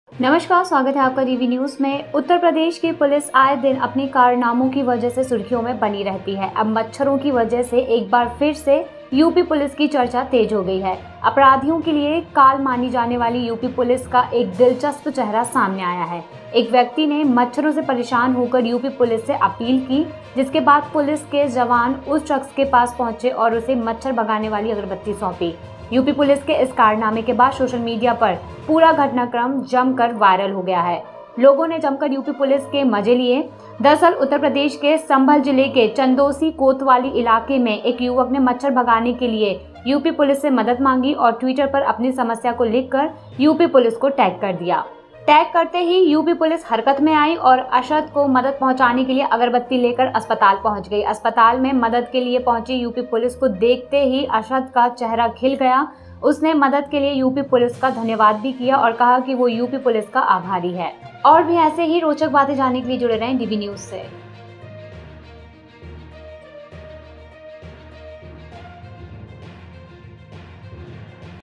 मच्छरों से परेशान शख्स ने UP Police से मांगी मदद, अगरबत्ती लेकर पहुंचे पुलिस के जवान | UP News
उत्तर प्रदेश की पुलिस आए दिन अपने कारनामों की वजह से सुर्खियों में रहती है. अब मच्छरों की वजह से एक बार फिर यूपी पुलिस की चर्चा तेज हो गई है. अपराधियों के लिए काल मानी जाने वाली यूपी पुलिस का एक दिलचस्प चेहरा सामने आया है. एक व्यक्ति ने मच्छरों से परेशान होकर यूपी पुलिस से अपील की, जिसके बाद पुलिस के जवान उस शख्स के पास पहुंचे और उसे मच्छर भगाने वाली अगरबत्ती सौंपी.

The police of Uttar Pradesh remain in headlines due to their exploits. Now due to mosquitoes once again the discussion of UP Police has intensified. An interesting face has come to the fore of the UP Police, which is considered to be the time for criminals. Distressed by mosquitoes, a person appealed to the UP police, after which the police personnel reached the person and handed him a mosquito repellent incense stick.